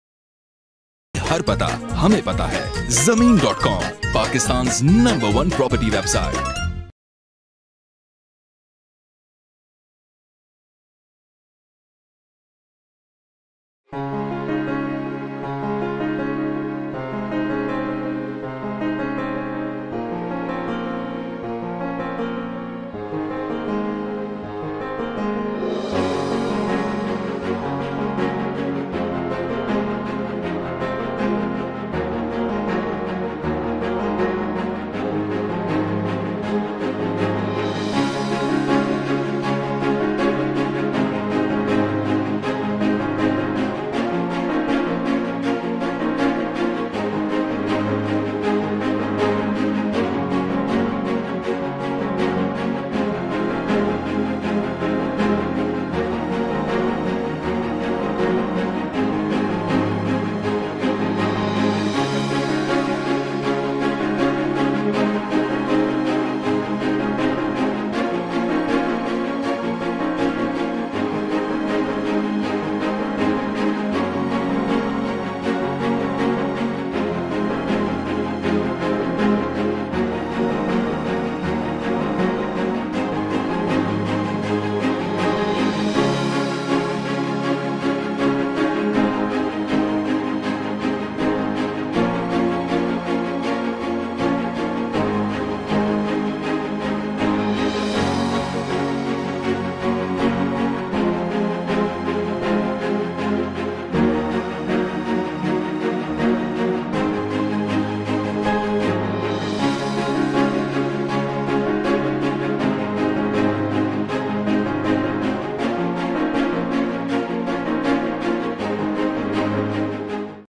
7 MARLA RESIDENTIAL PLOT FOR SALE IN BIN ALAM CITY KATAR BUND ROAD LAHORE
Amenities & Lifestyle:
• Sewerage
• Sui Gas
• Electricity
• Masjids
• Broadband Internet Access
• Satellite or Cable Tv Ready
• Maintenance Staff
• Security Staff
• CCTV Security
• Day Care Center
• Kids Play Areas
• Community Centers
• Lawn and Gardens
• Schools
• Shopping Malls
• Hospitals
• Restaurants
• Easy Access to Public Transport

Down Payment

Rs/ 795,000

48 Monthly Installments

Rs/ 26,145

8 Semi Annual payment

Rs/ 150,000

Balloting

Rs/ 350,000

Possession

Rs/ 500,000

Total Amount
Rs/ 3,950,000